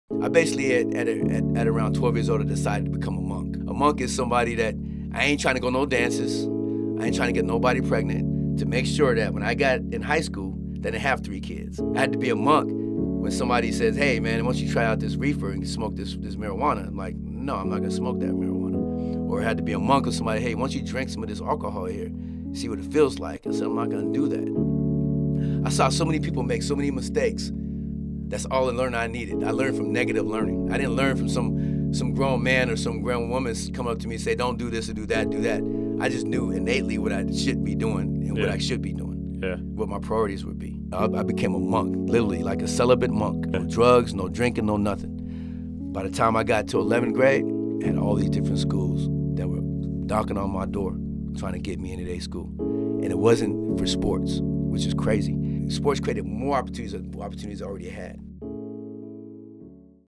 I DECIDED TO BECOME A MONK | TIM MANSON 🧘🏾‍♂️
Tim Manson on the decisions he had to make in order to stay out of trouble and become successful. Find the full episode on Apple Podcasts, Spotify, or on Youtube...